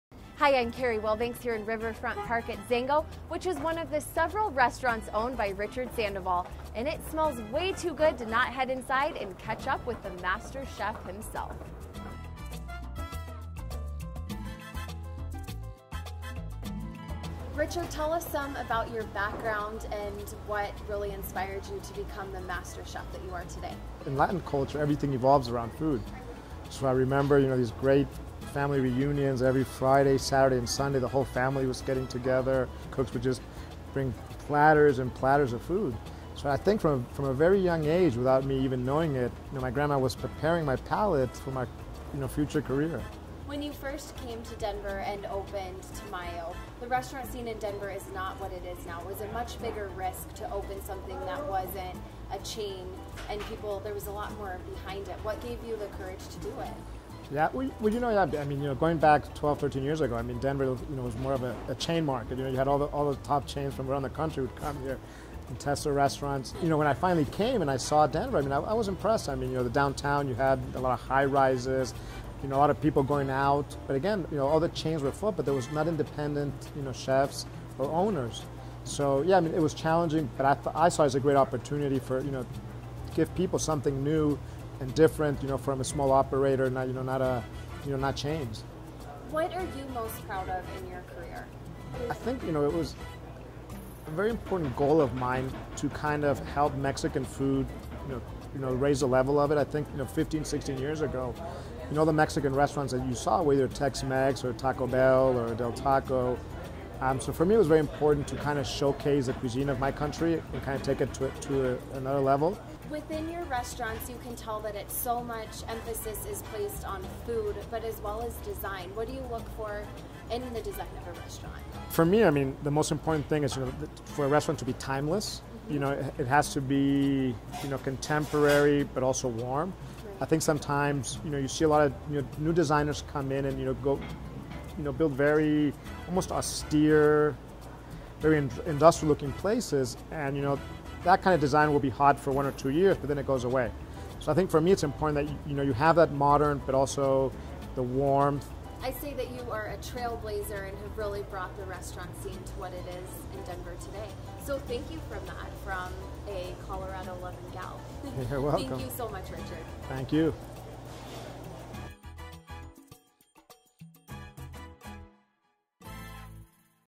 Catch Carri interviews Richard Sandoval
Filmed & Edited: Jason Pratt
Produced & Hosted: Carri Wilbanks

This is a video i made for Catchcarri.com.  We interviewed one of the largest names in the restaurant business Richard Sandoval at his restaurant Zengo.  The food was nice, but the space was simply amazing.